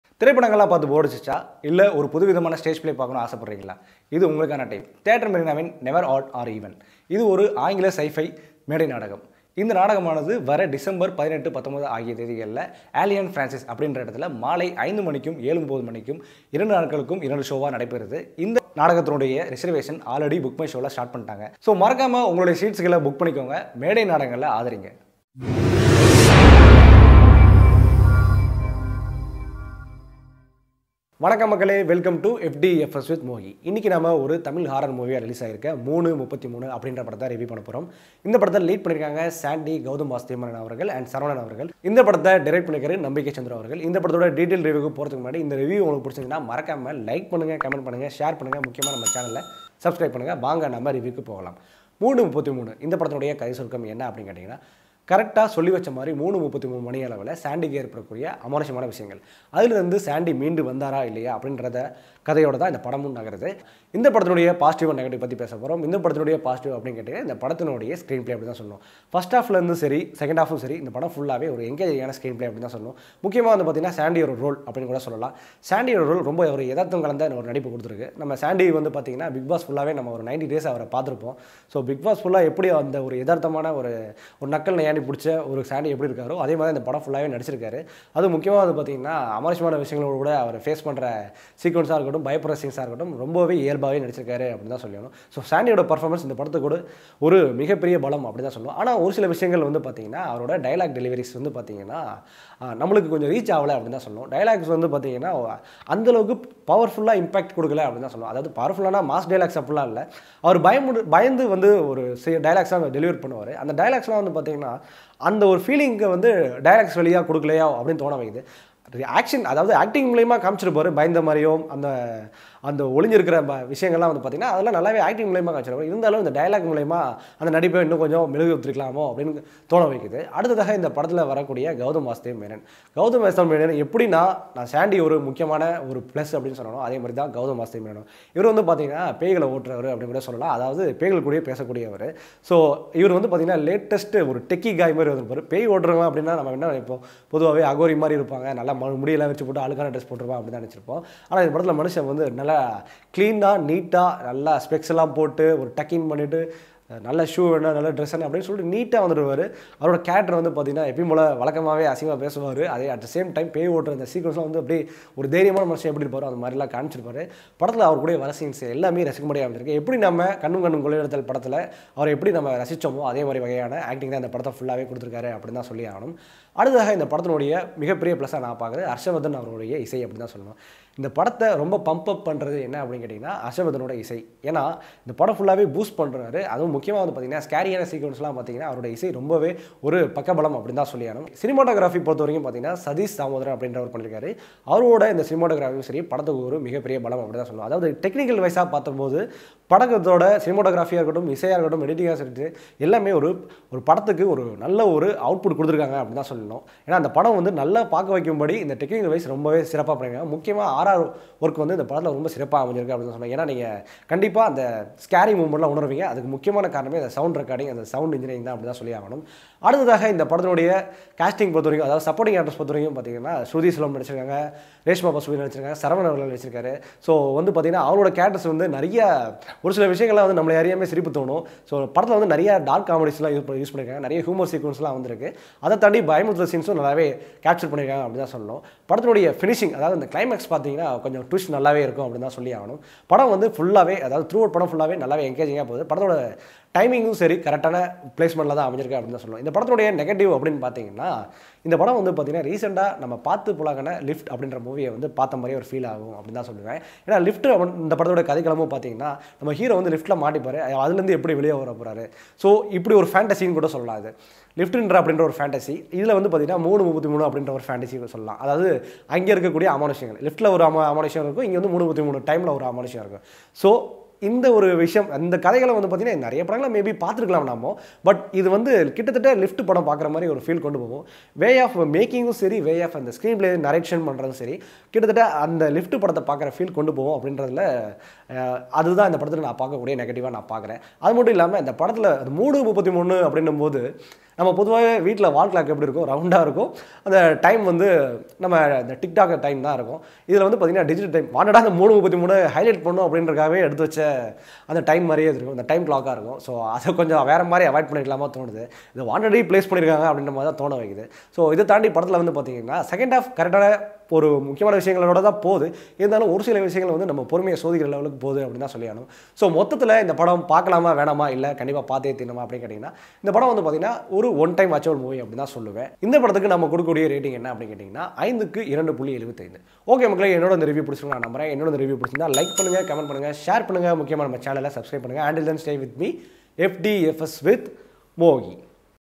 3:33 Movie Review | By Fdfs With Mogi | Sandy | Gautham Vasudev Menon | Nambikkai Chandru
Checkout my latest Tamil movie review

3:33

Starring Sandy Gautham Vasudev Menon

Please do watch and share to your friends and family members!

For more movie reviews and Reactions!